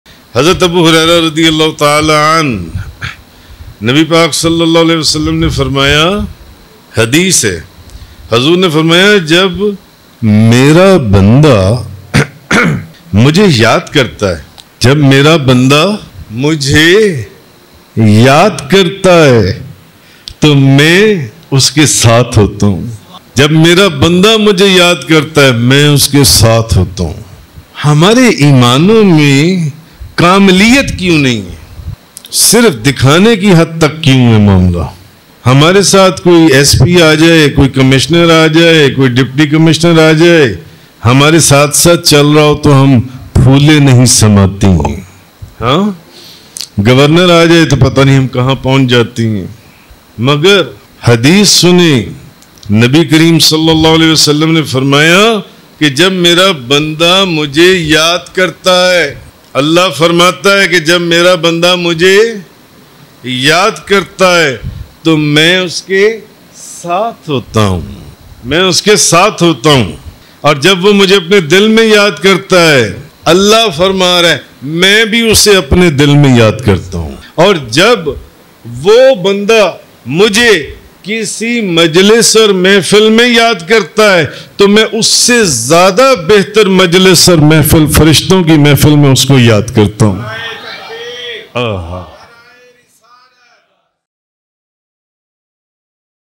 Allah Kis Ke Sath Hota Hai? | Al Shaykh Muhammad Mohsin Munawar Yousafi
*Description:*
This video explores a beautiful Hadith narrated by Hazrat Abu Hurairah  where the Prophet Muhammad ﷺ describes Allah's closeness to His servants.
The Hadith emphasizes how Allah responds with love and closeness to any effort made by His believers, whether they remember Him in their hearts, in gatherings, or strive to get closer to Him.
This powerful message reminds us of the importance of maintaining a strong relationship with Allah and the immense love and mercy He has for His devoted servants.

Clip # 03 Dated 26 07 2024.mp4 | Al Shaykh Muhammad Mohsin Munawar Yousafi